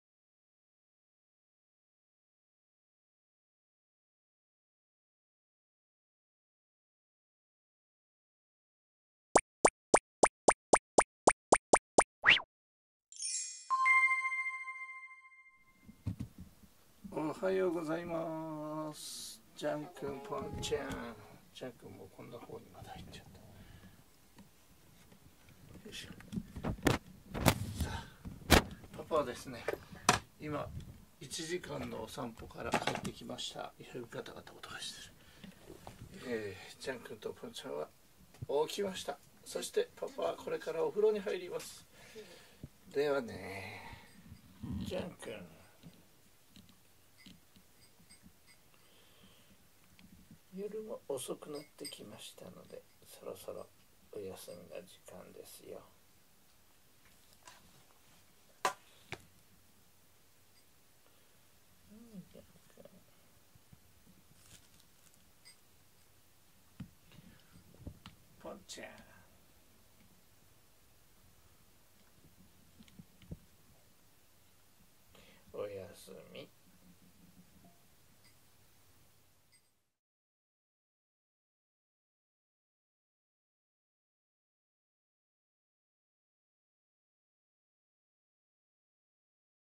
おはようからおやすみまで我が家は猫中心の生活です【Jean & Pont 3181】2023/4/9 保護猫ジャンけんポン
猫好きのうちはみんなそうでしょうけど、我が家も全て猫中心の生活になっています。
散歩に出かけるのも夫婦で時間をずらしてどちらかが家にいて、ジャンくんとポンちゃんが寂しくないようにと考えていたりします。
それでもここ最近は散歩の頻度も増えるとともに、仕事を得るための活動が活発化して猫中心が薄れているのです。
仕事が安定して健康を手に入れたらもっと猫中心にするぞ！